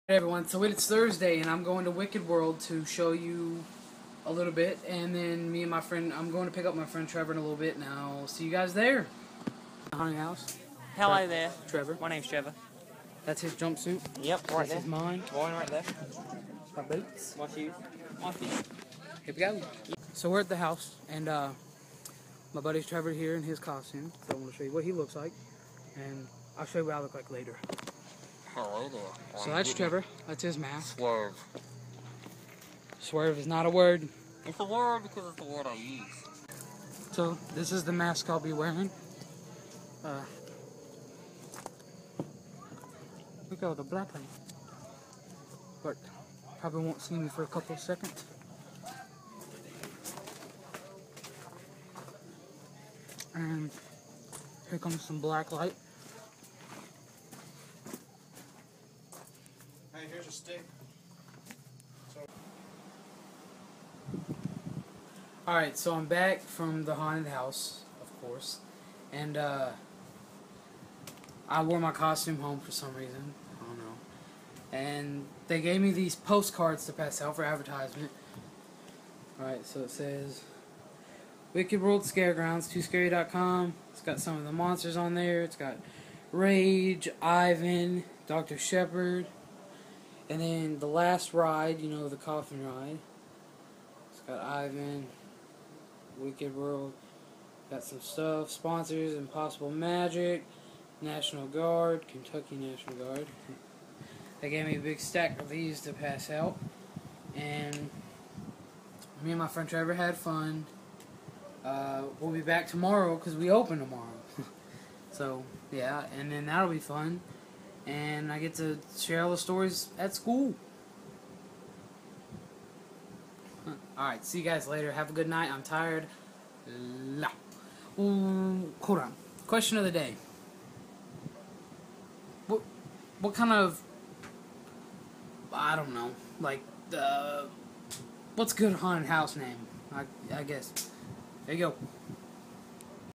At the haunted house!!!
I show you mine and my friend record costumes for the haunt.